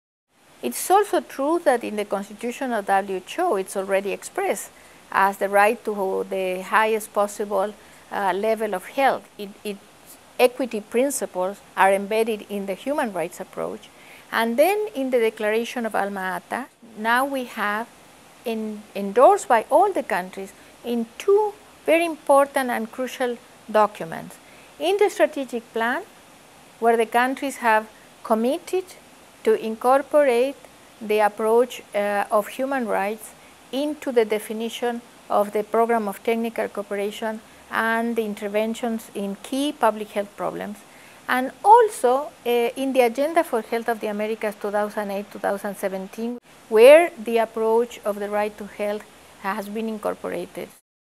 Dr. Mirta Roses, Director of the Pan American Health Organization
Dr. Roses, Director of the Pan American Health Organization, explains the Right to Health as established by the WHO Constitution and the Health Agenda for the Americas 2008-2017